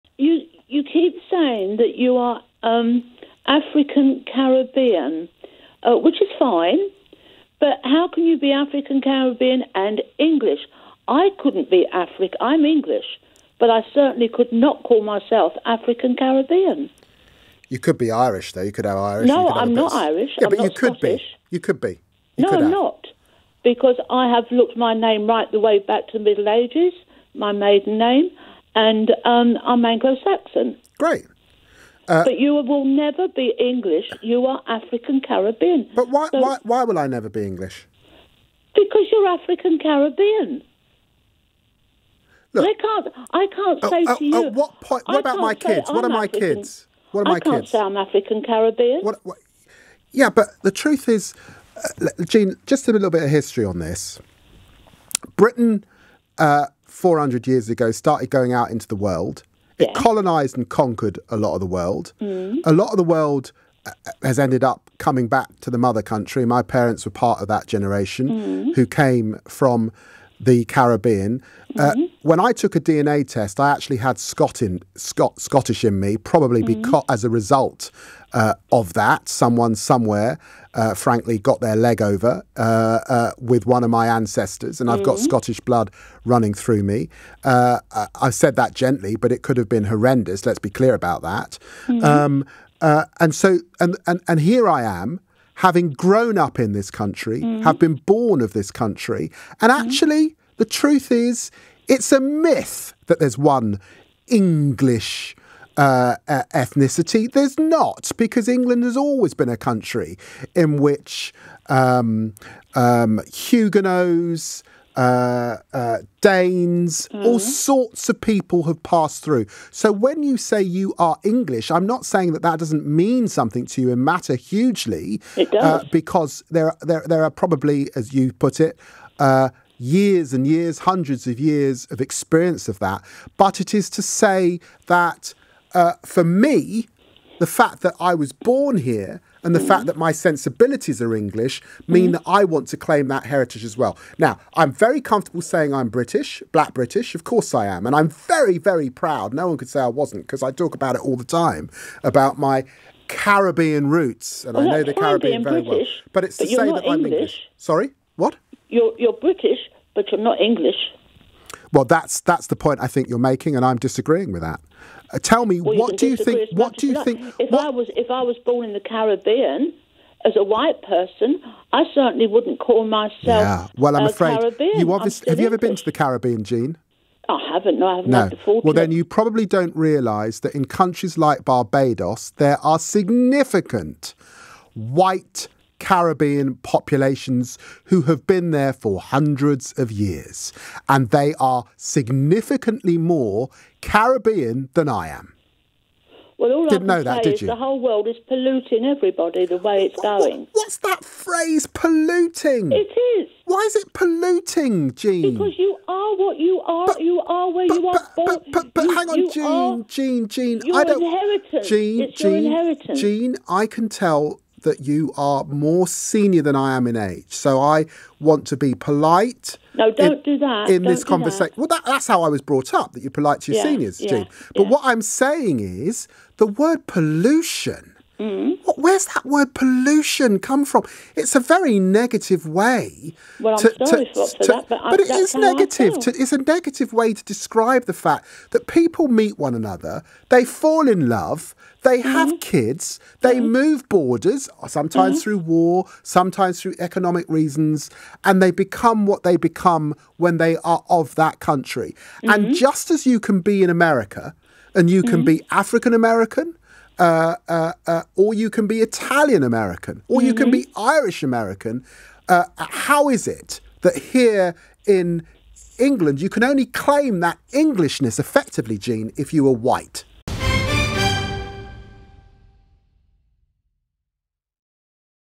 David Lammy challenges caller who tells him he's "not English" | LBC
David Lammy schools this caller who tells him he's "not English" because he's not white.